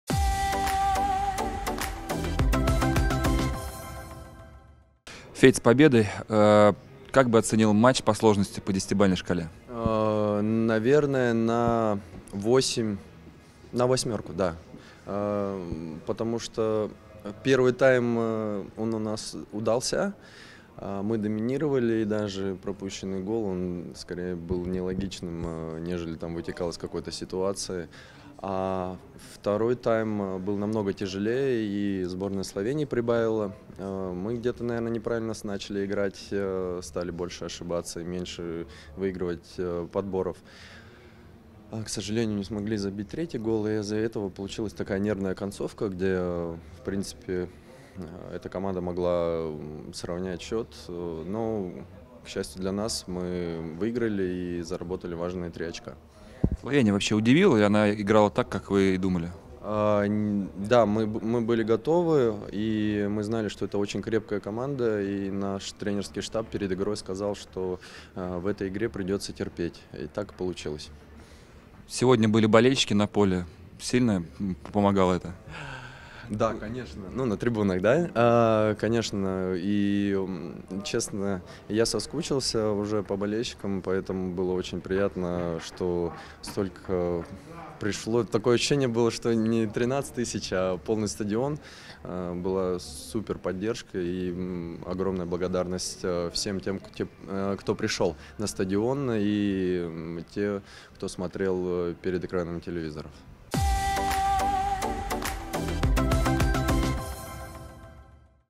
Фёдор Кудряшов: "Второй тайм получился намного тяжелее первого"
Защитник сборной России подвел итоги победного отборочного матча ЧМ2022 против Словении.